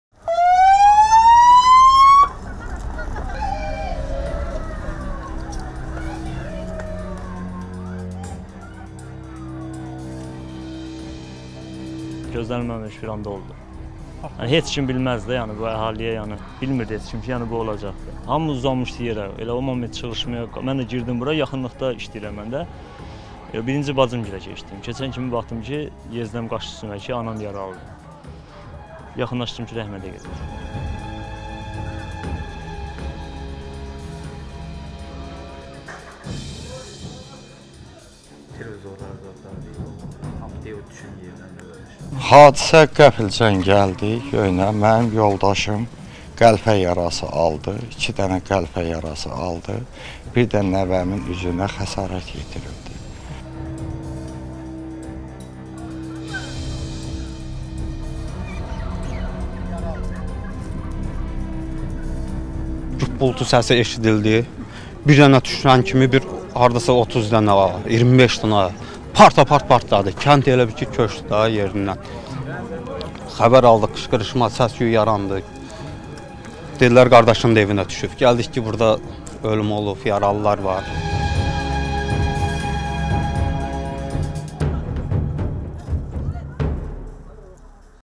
Düşmən Bərdəni kasetli raketlərlə atəşə tutub, ölən və yaralananlar var
Ermənistan silahlı qüvvələri humanitar atəşkəsi kobudcasına pozmaqda davam edir. Düşmən oktyabrın 27-də Azərbaycanın Bərdə rayonundakı dinc əhalini hədəf alaraq, yaşayış məntəqələrini mülki insanlara qarşı istifadəsi qadağan olunan kasetli raketlərlə atəşə tutub.